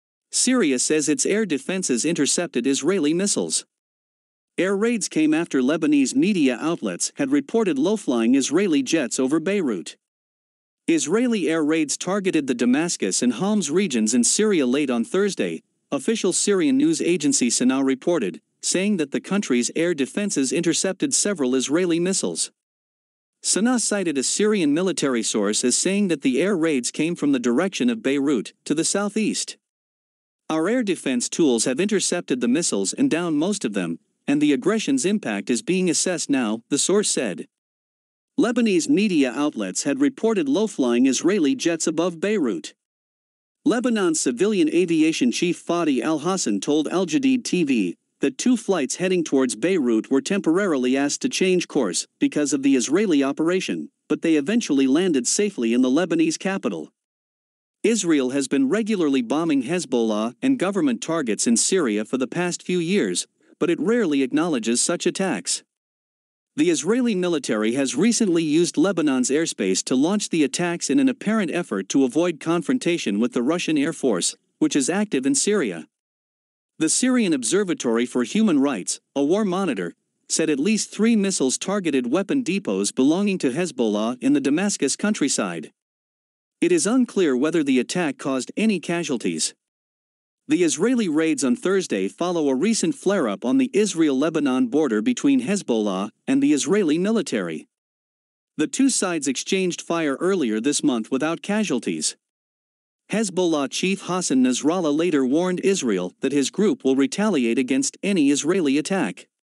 Syria says its air defences intercepted Israeli missiles
Air raids came after Lebanese media outlets had reported low-flying Israeli jets over Beirut.
Israeli air raids targeted the Damascus and Homs regions in Syria late on Thursday, official Syrian news agency SANA reported, saying that the country’s air defences intercepted several Israeli missiles.